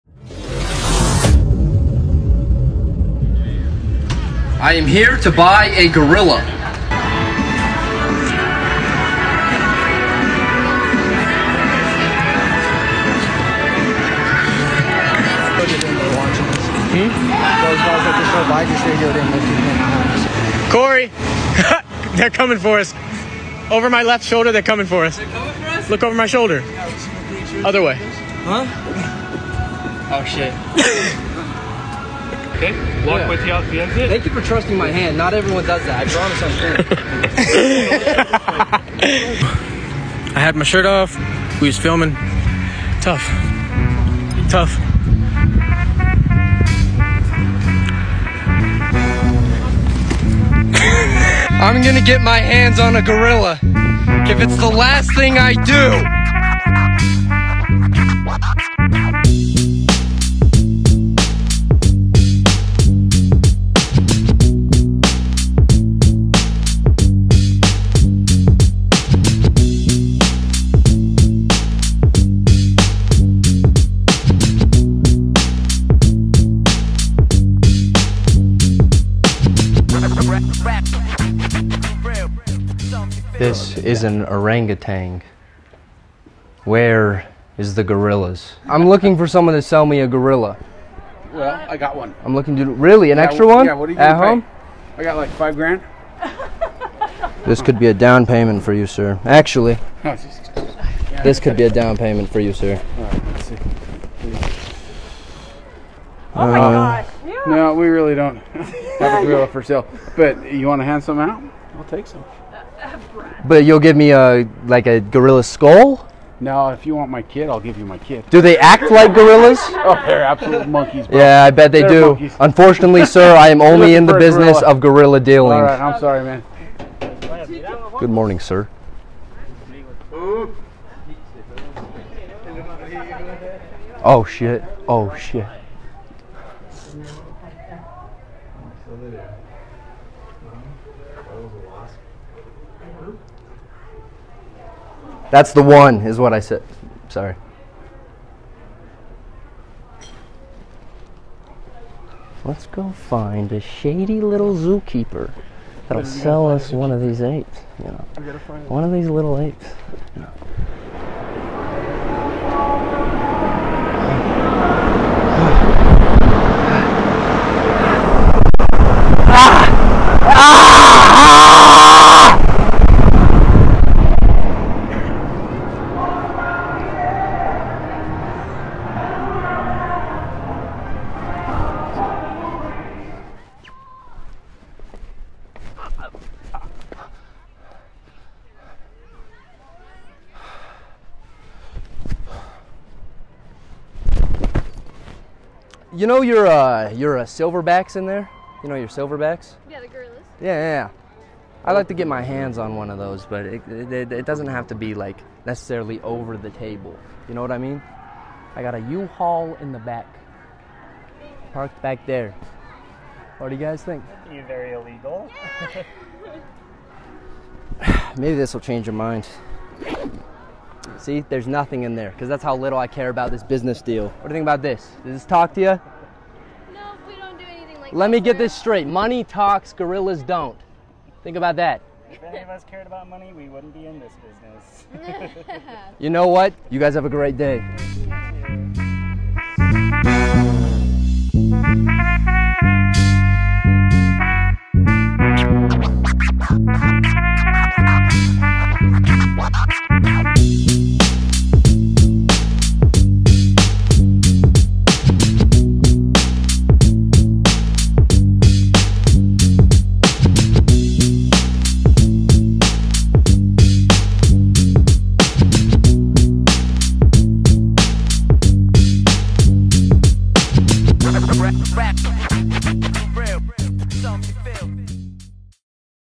I Tried To Steal A Gorilla From My Local Zoo (Again)
in this video I go to my local zoo (Utah's Hogle Zoo) & attempt to make a zookeeper an offer her can't refuse ($800) in exchange for an adult gorilla